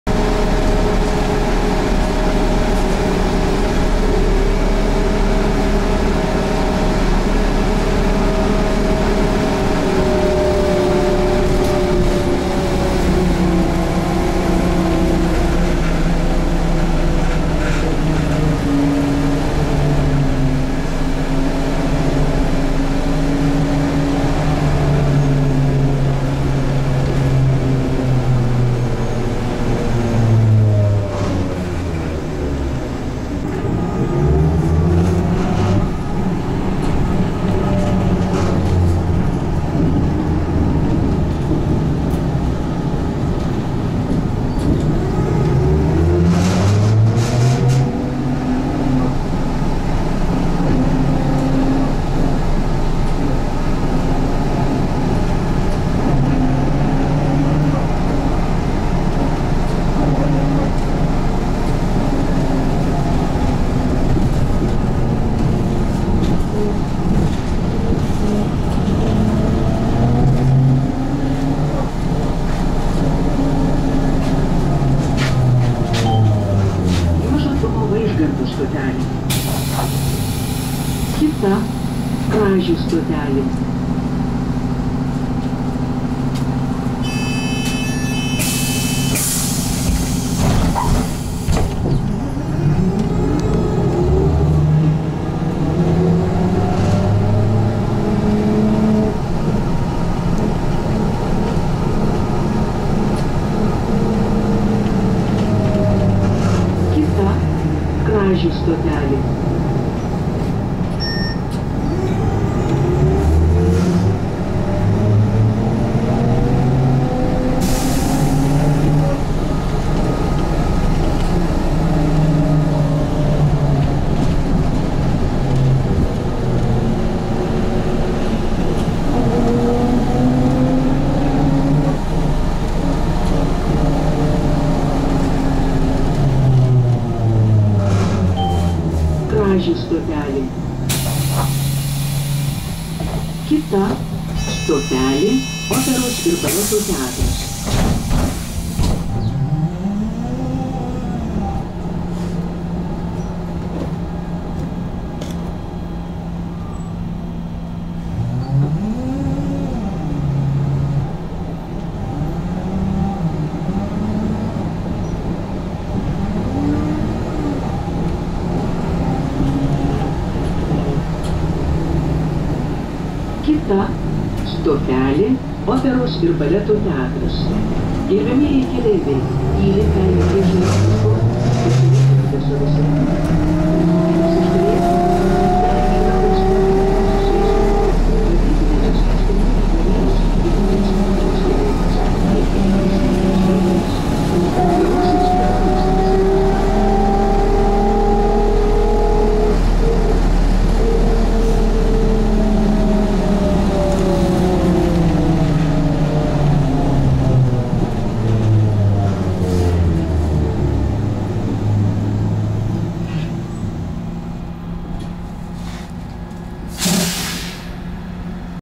Trolleybus Vilnius Lithuania Sony FDR-X300 4K November 2018!
We drive an old trolley bus in Vilnius city country Lithuania. Movies with Sony FDR-X3000 Action Camera In 4K.